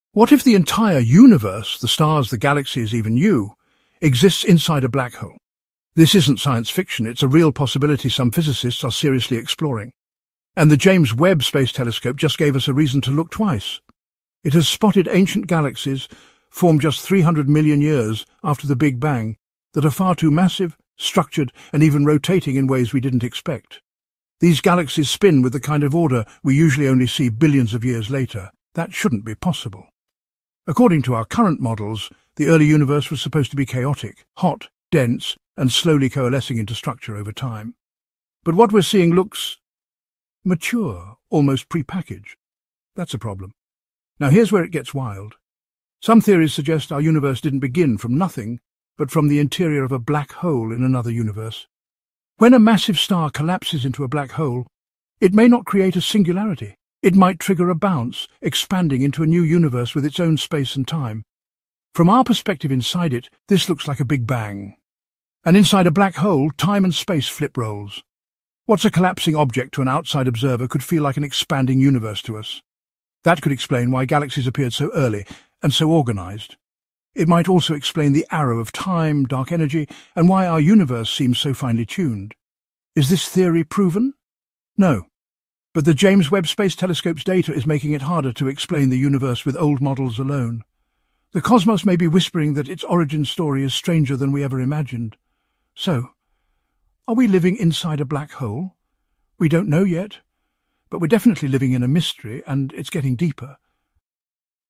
Are We Living In A Blackhole?
Could our entire universe be living inside a black hole? New discoveries from the James Webb Space Telescope reveal massive, well-structured galaxies forming much earlier than expected—challenging our understanding of cosmic history. Some physicists are now exploring the extraordinary idea that our universe might have originated from inside a black hole in another universe. Dive into the mysteries of dark energy, the arrow of time, and the fine-tuning of the cosmos. Is this science fiction—or the next frontier of physics? Watch and decide.